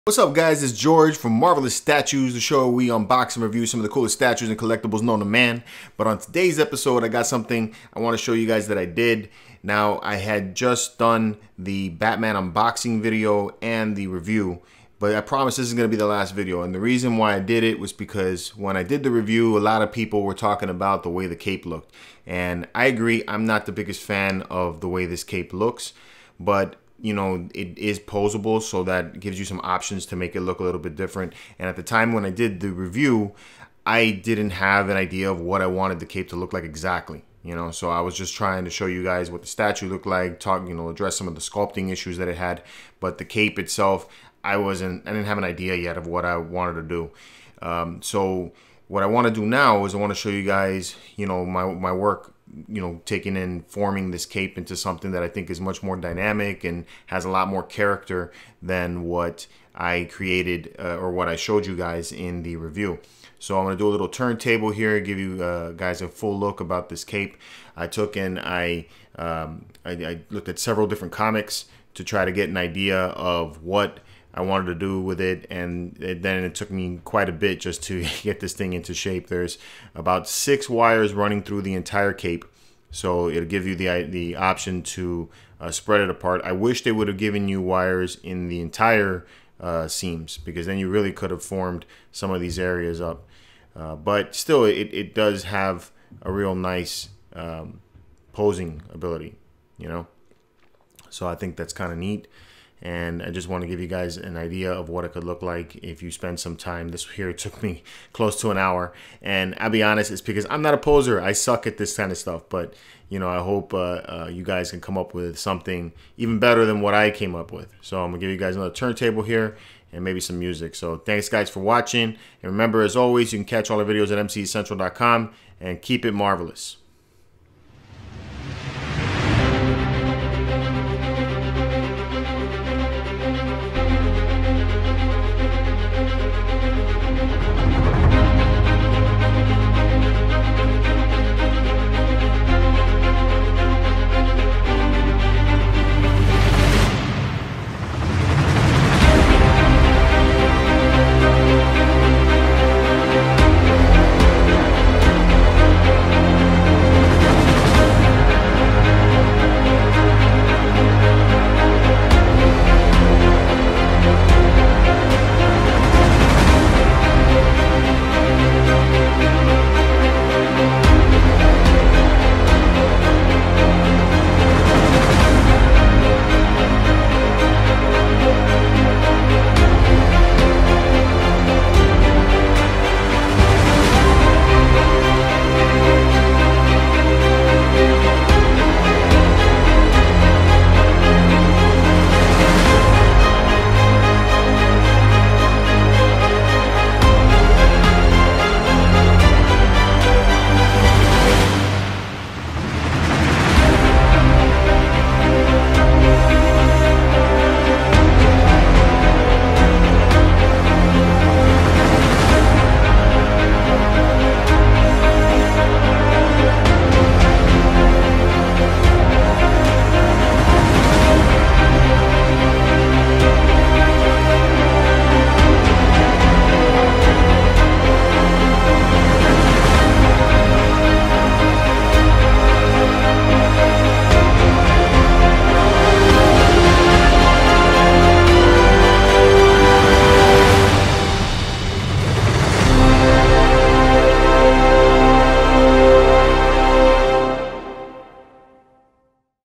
Sideshow Batman Premium Format Figure Cape Posing
After seeing many comments about the look of Batman's cape, I decided to do a video showing the cape posing possibilities.

Here is the Sideshow Batman Premium Format Cape Posing video.

If you want to buy the Batman Premium Format Figure Exclusive Statue, it is available at Sideshow Collectibles